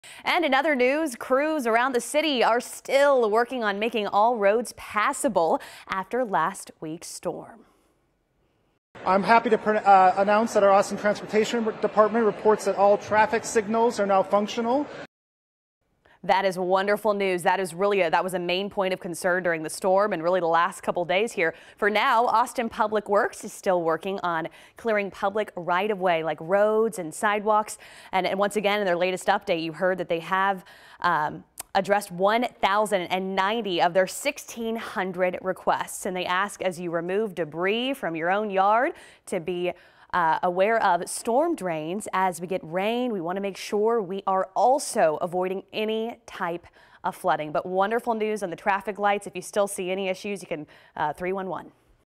All Austin traffic lights functioning after winter storm | KVUE
Crews around Austin are still working to make all roads passable after last week's storm. But all traffic signals are now functional.